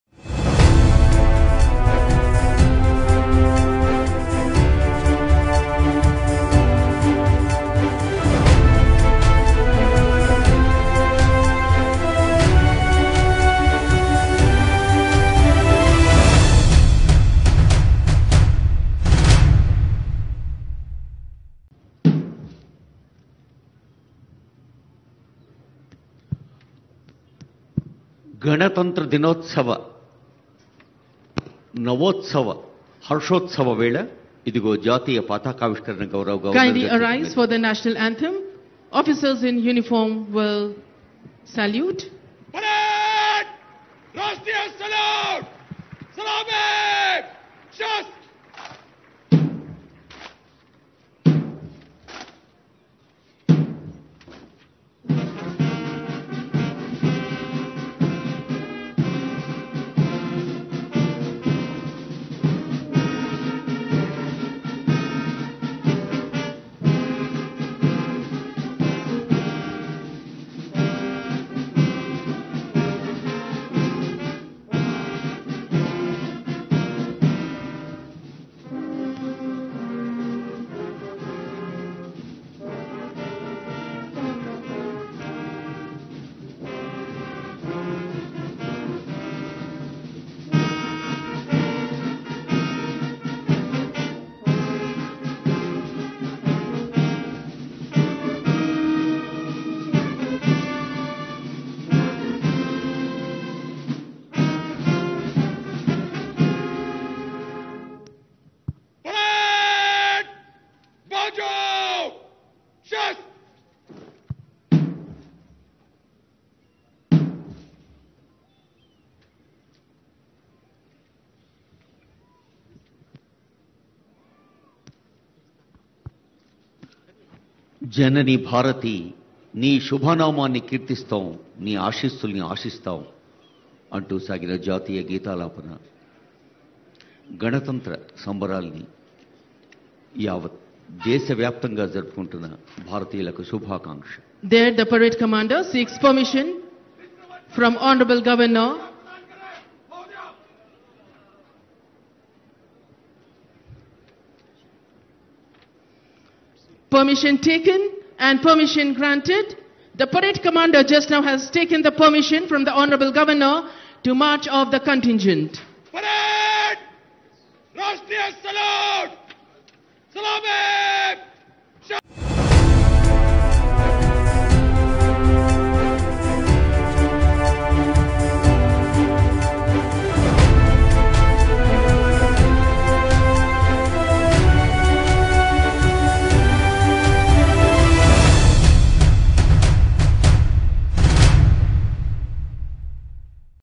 రిపబ్లిక్ డే వేడుకల్లో పాల్గొన్న తెలంగాణ గవర్నర్..
Watch Live updates with "BIJ NEWS"
మహోన్నతమైన మన రాజ్యాంగాన్ని రాజ్యాంగకర్తలు ఎంతో ముందు చూపుతో తయారు చేశారని తెలంగాణ గవర్నర్ తమిళిసై సౌందరరాజన్ అన్నారు. గణతంత్ర దినోత్సవాన్ని పురస్కరించుకుని రాష్ట్ర ప్రజలకు శుభాకాంక్షలు తెలిపారు. హైదరాబాద్ నాంపల్లి పబ్లిక్ గార్డెన్స్‌లో గవర్నర్ జాతీయ పతాకాన్ని ఆవిష్కరించారు. తొలుత గవర్నర్‌కు సీఎం రేవంత్ రెడ్డి, సీఎస్ శాంతికుమారి, ఇతర అధికారులు స్వాగతం పలికారు. అనంతరం గవర్నర్..సైనిక దళాల గౌరవ వందనం స్వీకరించారు.